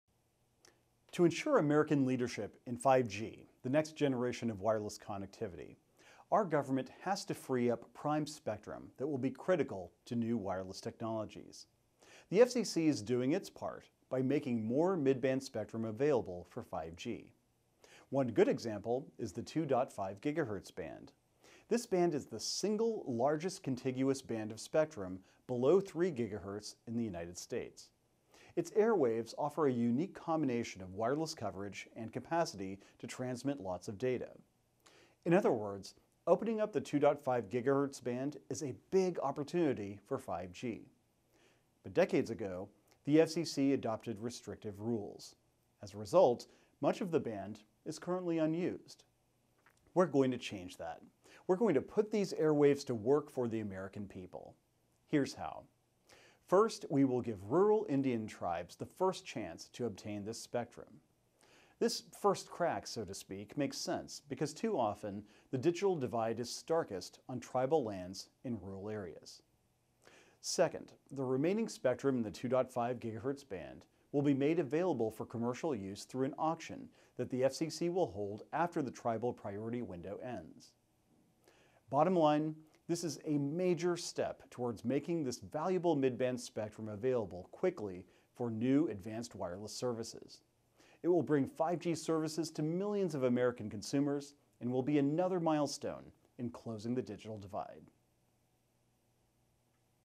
Freeing Up Unused Mid-Band Spectrum for 5G & Prioritizing Tribal Access
Chairman Pai outlines the FCC's decision to modernize the outdated regulatory framework for the 2.5 GHz band, making portions of this swath of vital mid-band spectrum available for advanced wireless services including 5G.  It establishes a priority filing window for rural Tribal Nations to provide them with an opportunity to obtain unassigned 2.5 GHz spectrum to address the communications needs of their communities.